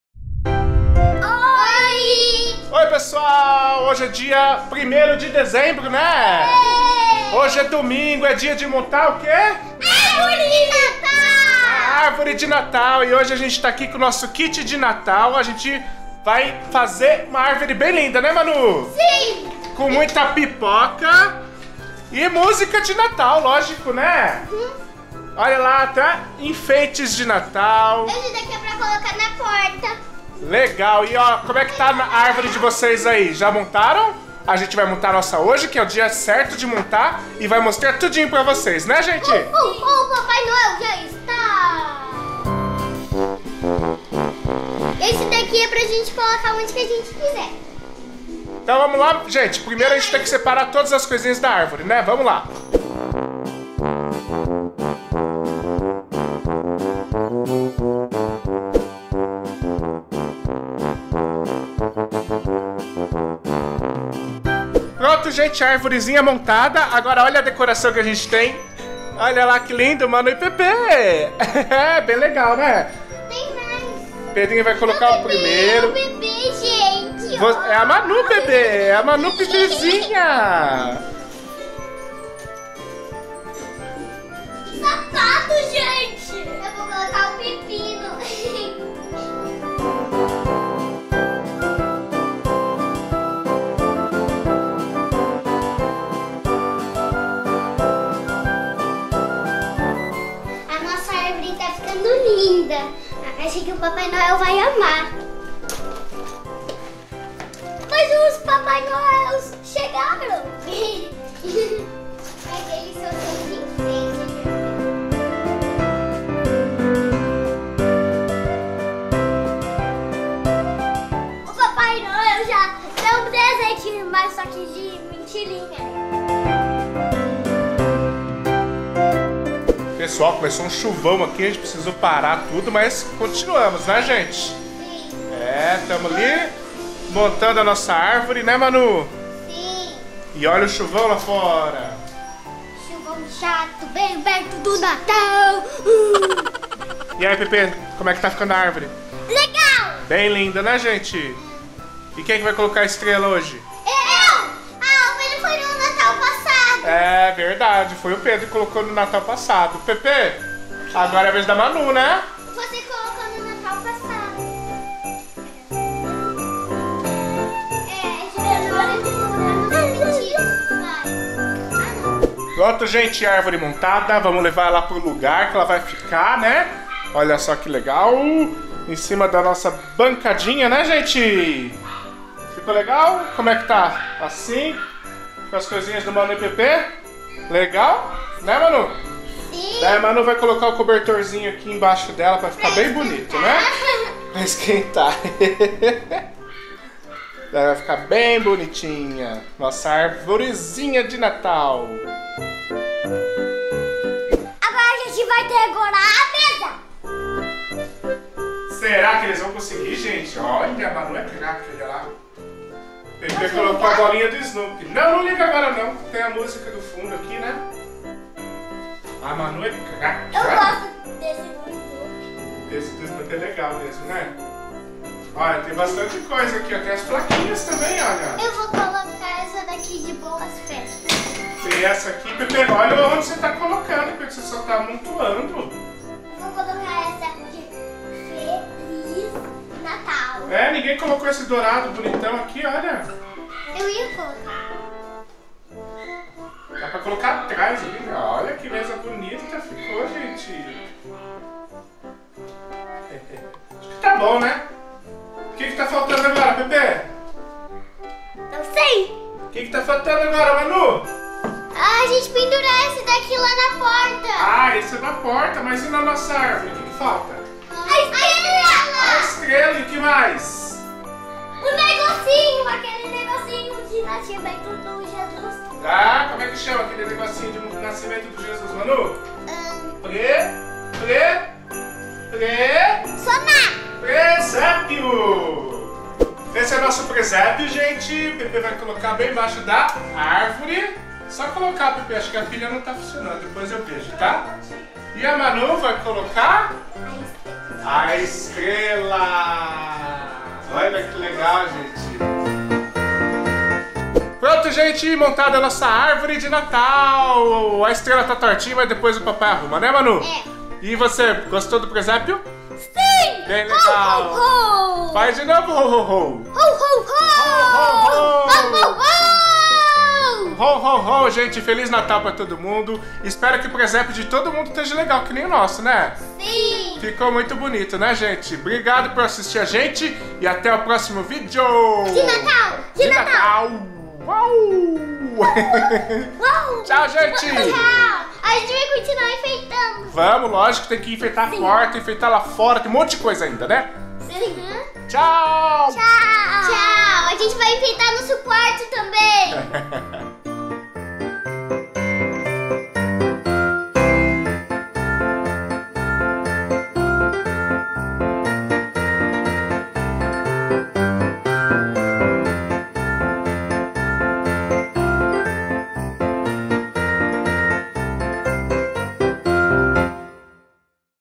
Montando a Árvore de Natal
Vamos montar nossa Árvore de Natal. E vocês? Já montaram as de vocês?
Vai ser SUPER legal.

Cartinhas & Presentes:

📦 Caixa Postal 4031 CEP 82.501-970 Curitiba / PR

👉 Canal Infantil: Livre para todas as idades, administrado e monitorado pelos responsáveis.

Todos os ítens mostrados foram comprados com recursos próprios.